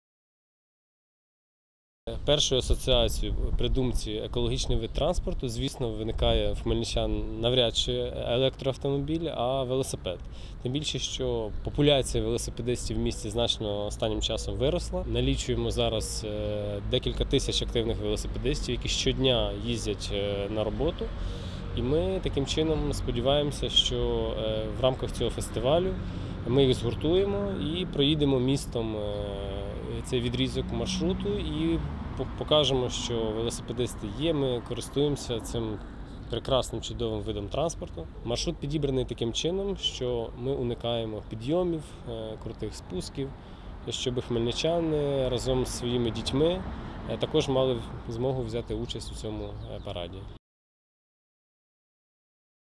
Зелений велопробіг 1
Заїзди на двоколісному відбудуться 7 травня в рамках фестивалю «ЕкоДрайвФест», тематика  якого – енергоефективність і екотренди 2016 року.